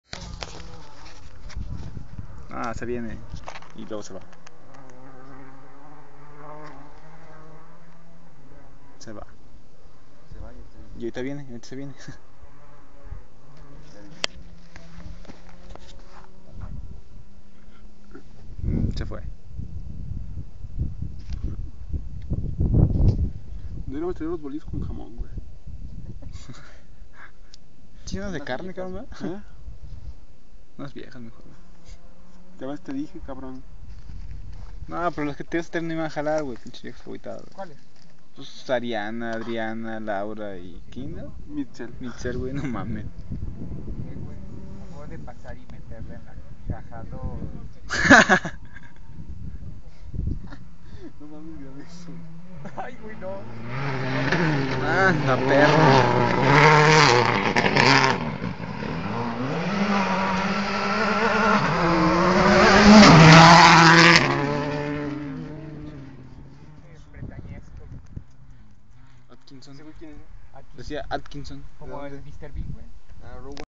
WRC 2008 Mexico Leon Guanajuato Corona Rally by 4 M.D.'s 27
The last WRC stage in Mexico, until 2010. This is Ibarrilla / Agua Zarca / El Zauco Stage (I think so,if you know its another one, let me know)
El ultimo rally en Mexico, hasta 2010, si alguien sabe que tramo es este (no sabemos como chingaos llegamos, pero llegamos)